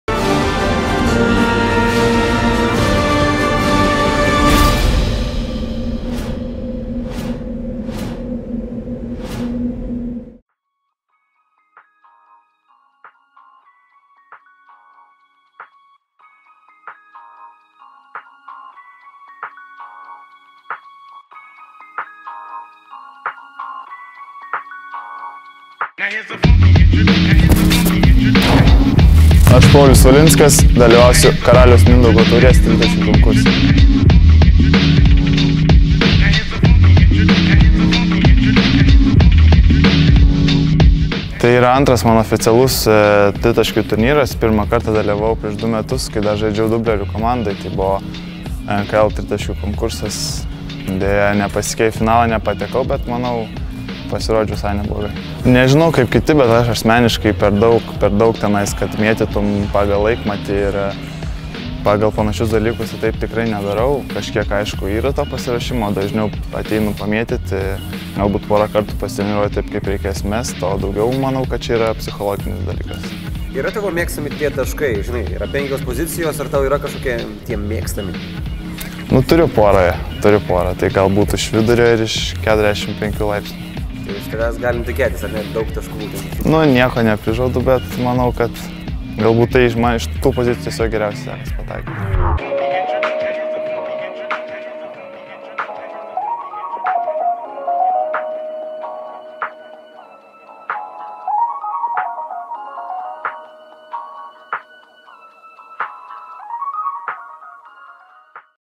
P.Valinskas apie KMT tritaškių konkursą: „Čia daugiau lemia psichologiniai dalykai“
Kauno „Žalgirio“ gynėjas Paulius Valinskas prieš artėjantį KMT snaiperių konkursą su „Žalgiris TV“ kalbėjo apie tritaškius metimus bei patirtį tokiuose konkursuose.